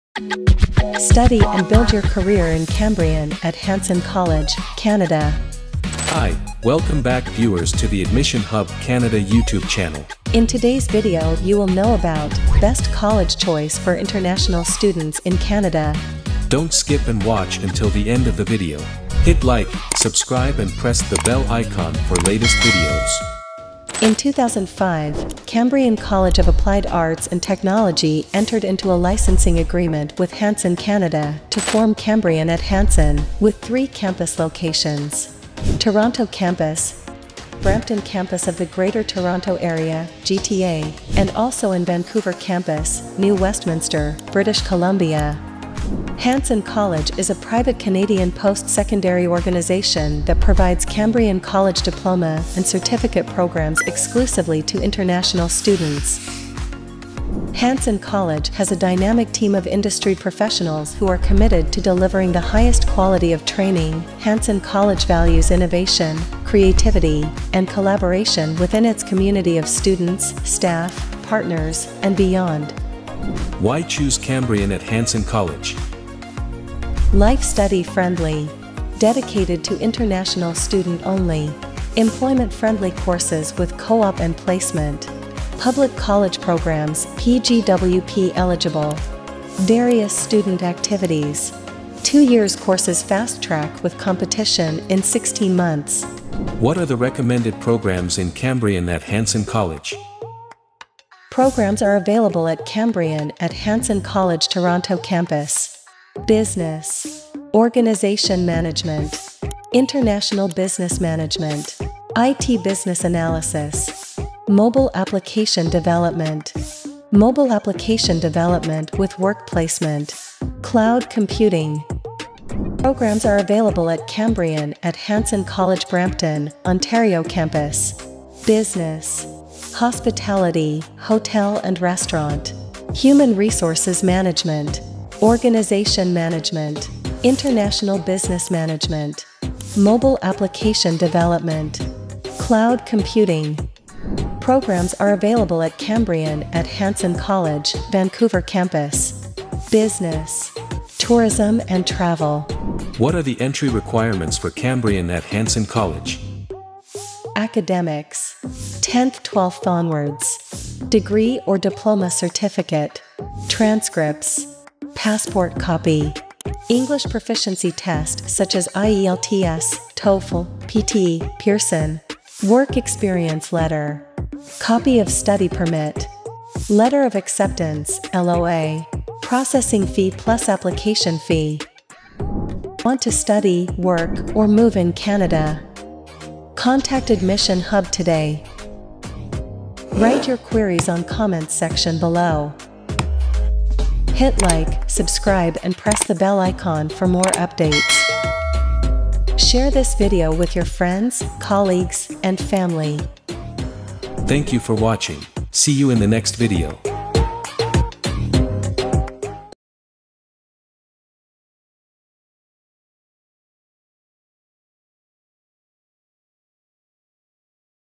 "Unlock Your Potential - Why Study at Cambrian College@Hanson."
In 2005, Cambrian College of Applied Arts and Technology entered into a licensing agreement with Hanson Canada to form Cambrian at Hanson. With three campus locations, Hanson delivers a selection of Cambrian College programs exclusively to international students.

Canada continues to rank among one of the best countries in the world as a place to study, live, and work! The experience of studying abroad in Canada offers international students the opportunity to receive advanced training and a future in Canada due to the value of Canadian education credentials.

Join us in shaping tomorrow's leaders and innovators, starting with your journey at this Precious College ."